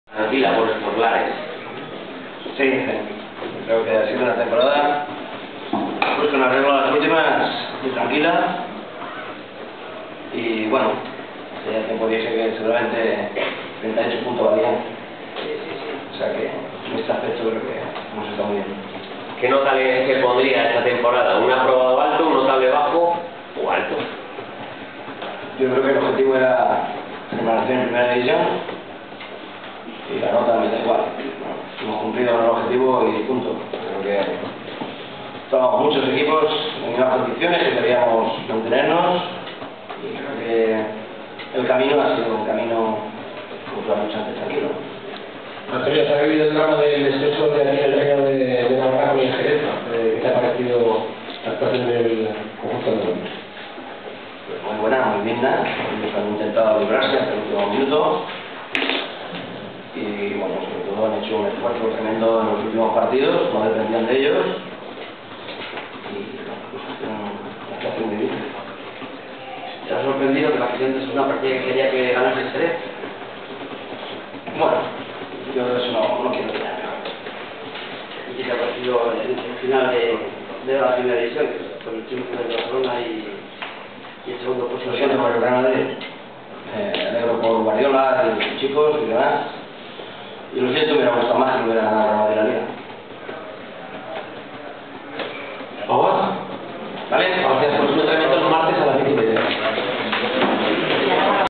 Osasunista.net
Camacho hace un resumen del empate ante el Xerez (1-1) en el Sadar, en el último partido de liga.